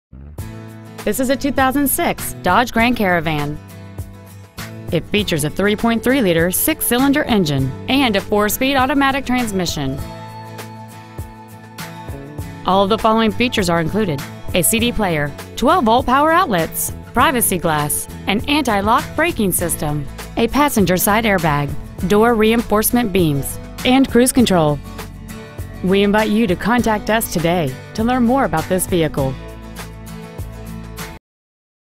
2006 DODGE GRAND CARAVAN Granbury TX
Year : 2006

 Make : DODGE

 Model : GRAND CARAVAN

 Engine : 3.3L V6 12V MPFI OHV

 Trans . : 4-SPEED AUTOMATIC

 Exterior : GRAY

 Miles : 104,278

 SouthWest Ford

 800-NEW-FORD

 3000 Ft Worth Hwy

 Used 2006 DODGE GRAND CARAVAN  TX